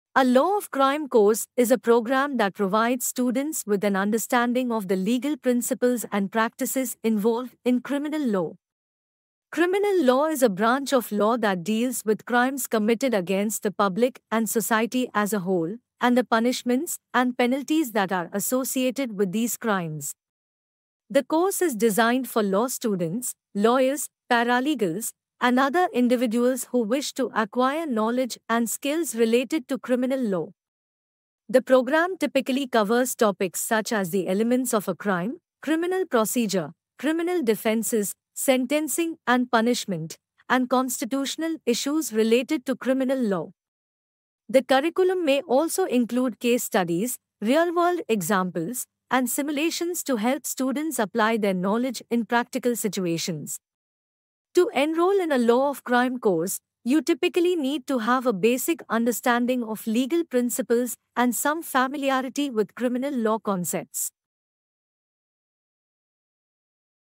Law of Crime Course
A Law of Crime course is a program that provides students with an understanding of the legal principles and practices involved in criminal law. Criminal law is a branch of law that deals with crimes committed against the public and society as a whole, and the punishments and penalties that are associated with these crimes.

The course is designed for law students, lawyers, paralegals, and other individuals who wish to acquire knowledge and skills related to criminal law. The program typically covers topics such as the elements of a crime, criminal procedure, criminal defenses, sentencing and punishment, and constitutional issues related to criminal law. The curriculum may also include case studies, real-world examples, and simulations to help students apply their knowledge in practical situations.

To enroll in a Law of Crime course, you typically need to have a basic understanding of legal principles and some familiarity with criminal law concepts. The course may be offered online or in-person, and may range from a few weeks to several months in duration.